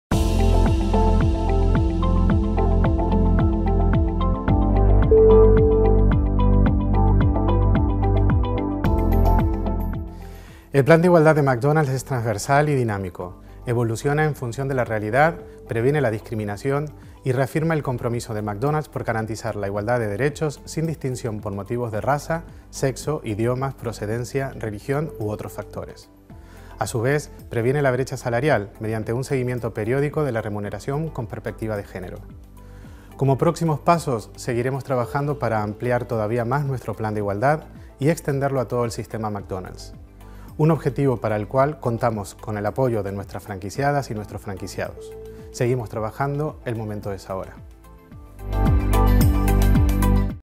Plan de Igualdad de McDonald´s, Diego Trombatore
El certificado de Igualdad de Género de AENOR representa un reconocimiento a la labor desarrollada por Recursos Humanos de McDonald´s, donde se indica que se esta trabajando en el caminio adecuado, creando un buen entorno de trabajo, inclusivo y respetuoso